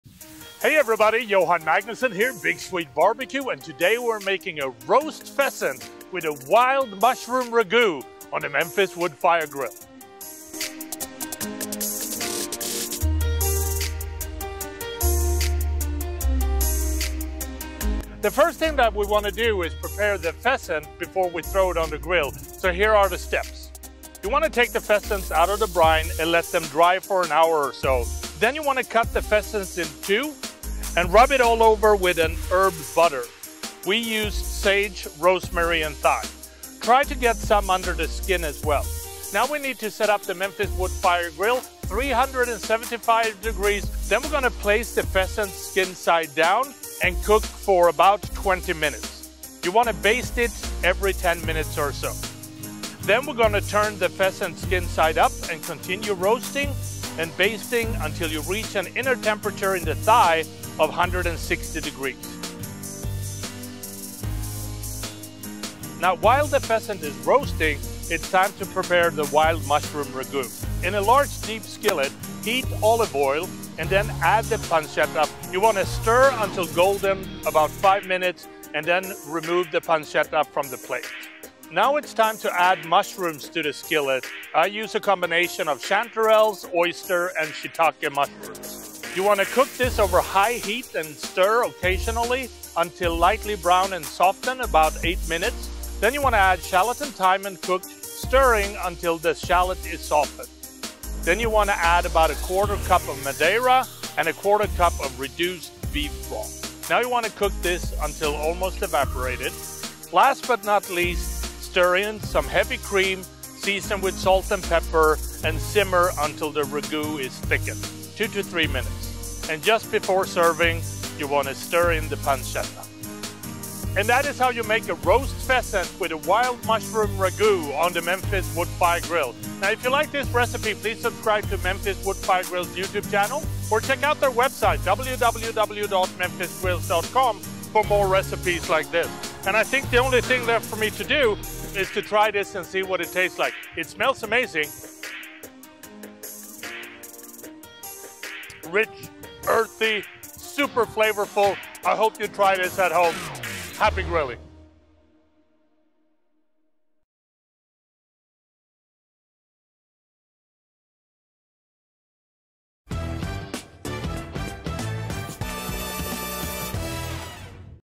Roast Pheasant with a Wild Mushroom Ragu
A rich, earthy and flavorful wild mushroom ragu´ is the perfect enhancement to pheasant. Big Swede BBQ shows you his version of a delicious pheasant dish!

Pheasant

2 whole pheasants
2 cups of Herbed Butter (Sage, Rosemary, Thyme)

Brine

1/4 cup kosher salt, about 2 1/4 ounces
1/4 cup brown sugar
4 cups water
10 Black Pepper Corns
4 Bay leaves

Wild Mushroom Ragu

1 teaspoon extra-virgin olive oil
1/4 lb thinly sliced pancetta, coarsely chopped
1 ½ lbs mixed mushrooms, such as chanterelles, porcini, oyster, shiitake
1 large shallot, finely chopped
1 tsp finely chopped thyme
1/4 cup Madeira Wine
1/4 cup beef or veal reduction
3/4 cup heavy cream
Salt and freshly ground pepper

Preparation

Dissolve the salt and sugar in the water and add the peppercorns and bay leaves. Find a lidded container just about large enough to hold both pheasants. Cover them with the brine and let this sit in the fridge for at least 12 hours and up to 18 hours.

Take the pheasants out of the brine. Set in a breezy place and let them dry for an hour or so. This drying process is an important step. Cut the pheasants in two and rub with the herbed butter thoroughly. Try to get some under the skin as well. Reserve half of the herbed butter for basting.

Set your Memphis Wood Fire Grill to 375 F using Apple Pellets.

Place the pheasant skin side down and cook for 30 minutes. Baste every 10 minutes. Then turn the pheasant skin side up and continue roasting and basting until you reach an inner temperature in the thigh of 160F.

While the pheasant is roasting, prepare the wild mushroom ragu. In a large deep skillet, heat the olive oil. Add the pancetta and cook over moderately high heat, stirring, until golden, about 5 minutes. Using a slotted spoon, transfer the pancetta to a plate, leave the fat in the skillet.

Add the mushrooms to the skillet and cook over moderately high heat, stirring occasionally, until lightly browned and softened, about 8 minutes. Add the shallot and thyme and cook, stirring, until the shallot is softened. Add the Madeira and the beef or veal reduction and cook until evaporated, scraping up any browned bits on the bottom of the skillet. Stir in the cream, season with salt and pepper and simmer until the ragu is slightly thickened, 2 to 3 minutes. Just before serving, stir in the pancetta.

Serve the ragu with the pheasants.